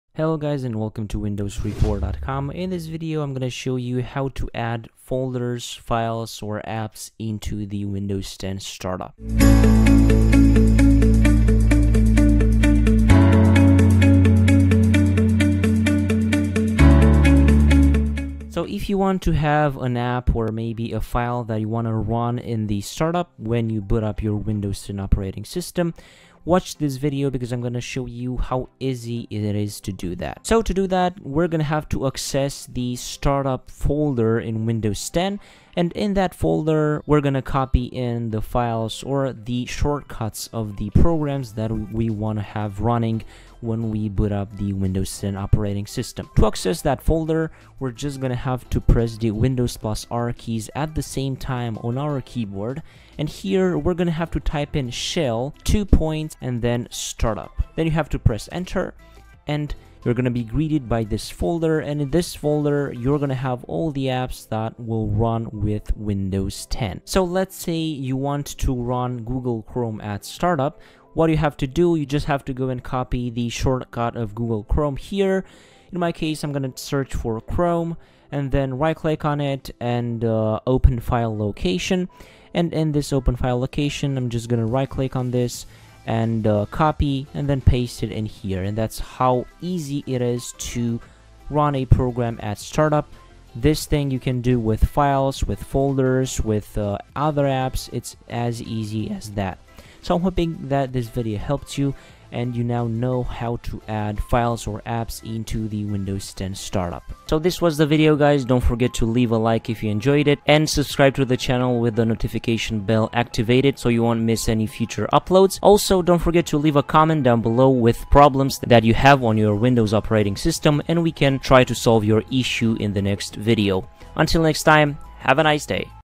How to ADD Files/Apps to Startup on Windows 10
| ⚙️Try Fortect to fix and optimize your PC like a Pro

In this video, we're going to show you How to ADD Files/Apps to Startup on Windows 10. Watch this video to see a very quick solution.

Solution : 0:17